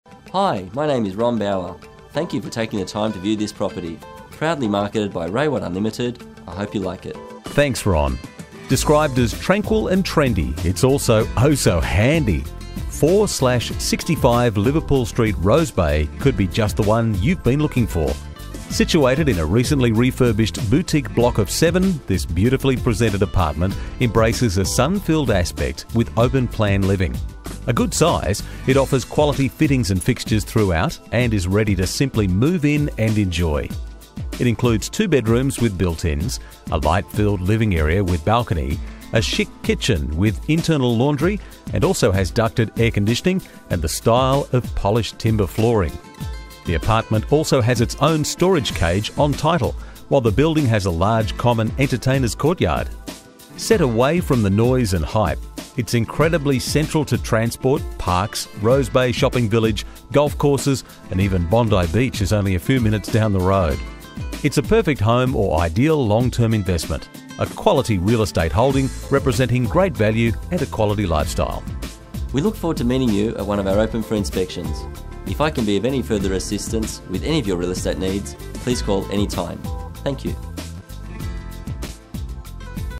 SOLD AT AUCTION!!! TRANQUIL & TRENDY - 4/65 LIVERPOOL ST, ROSE BAY
Situated in a refurbished boutique block, this beautifully presented apartment embraces a sun-filled aspect with open plan living. A good size, it offers quality fittings and fixtures throughout and is ready to simply move in and enjoy. Central to transport, parks, shopping village and beach, it is a perfect home or ideal long-term investment - a quality real estate holding

* 2 bedrooms with built-ins
* Light-filled living area with balcony
* Chic kitchen with int laundry
* Ducted air con, polished timber floors
* Security boutique block of 7
* Large common entertainers courtyard
* Storage cage on title
* Great value quality lifestyle

View: Saturday & Thursday 11am - 11.45am

Auction: Wednesday 16 November, 6.30pm
Club Rose Bay
Cnr Vickery Avenue & New South Head Road
Parking behind Lyne Park Tennis

Agent: Ron Bauer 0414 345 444